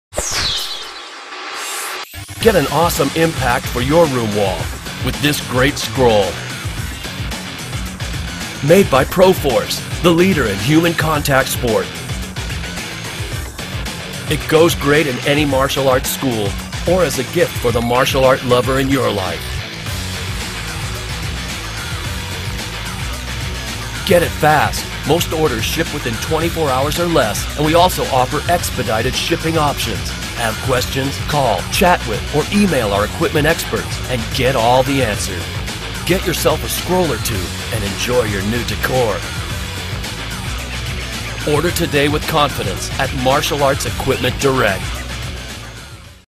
Wing Tsun Kung-Fu Poster - MAEQD.COM
Product Description:
Learn the art of Wing Tsun Kung-Fu as demonstrated by Grandmaster Leung
Ting himself.

Features:
* Learn the art of Wing Tsun Kung-Fu.
* Measures 23" x 24".
* 5 poster set includes demonstrations of Siu-Nim-Tau, Chum-Kiu, Biu-Tze,
Application of the Movements and Wooden Dummy Techniques.